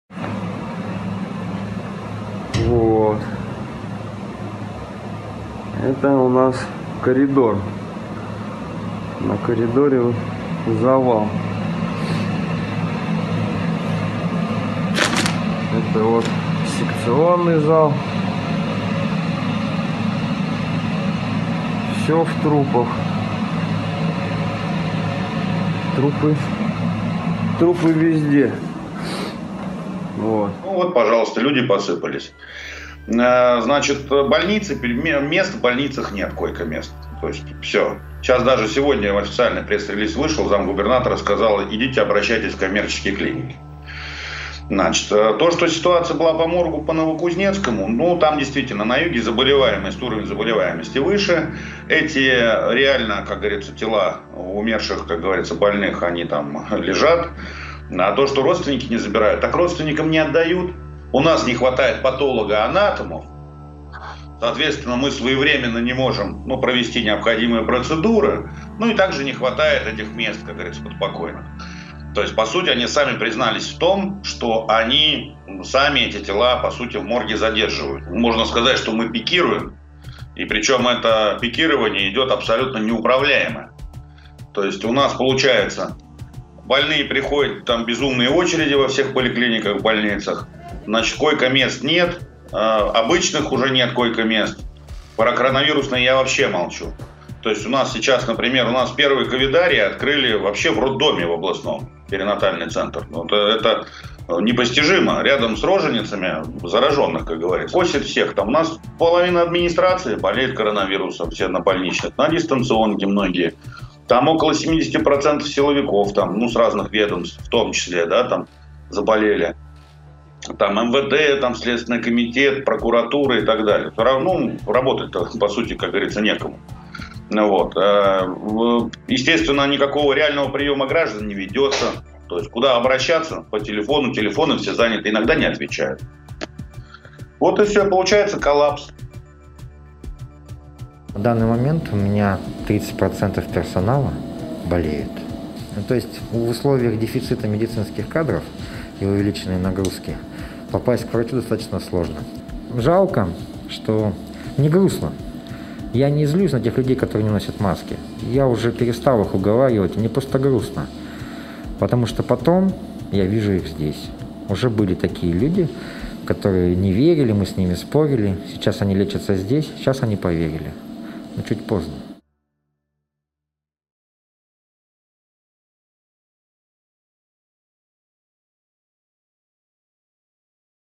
“Всё в трупах”. Родственники умерших не могут забрать тела из морга
Родственники умерших от COVID-19 в Новокузнецке не могут забрать тела из морга.